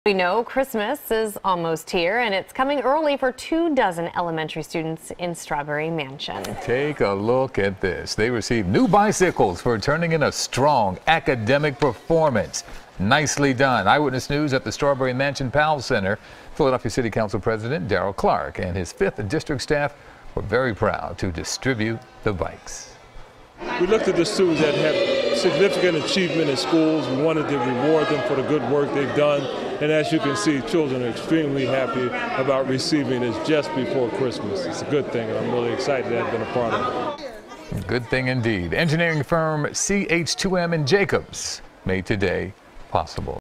Christmas Came Early For Two Dozen Elementary Students In Strawberry Mansion
Students in Strawberry Mansion received new bikes for turning in a strong academic performance.